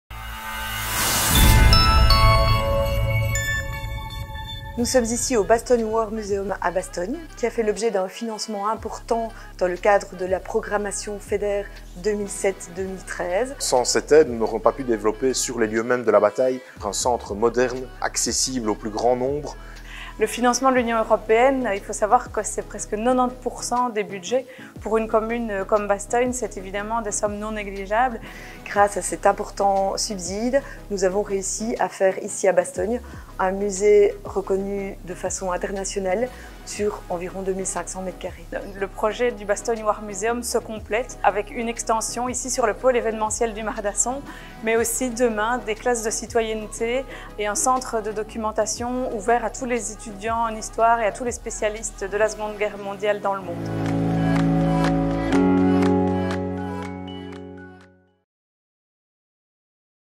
L’Europe est proche de vous
Saviez-vous que le Bastogne War Museum, géré par l’agence Tempora, est un projet cofinancé par l’Union européenne ?

Cette vidéo nous emmène à Bastogne où, grâce à l'Europe et à la Wallonie, le Bastogne War Museum a pu voir le jour et devenir un site de mémoire majeur dédié à la Seconde Guerre Mondiale.

L'Europe et les autorités publiques investissent dans plus de 1000 projets près de chez vous… et continueront évidemment à le faire !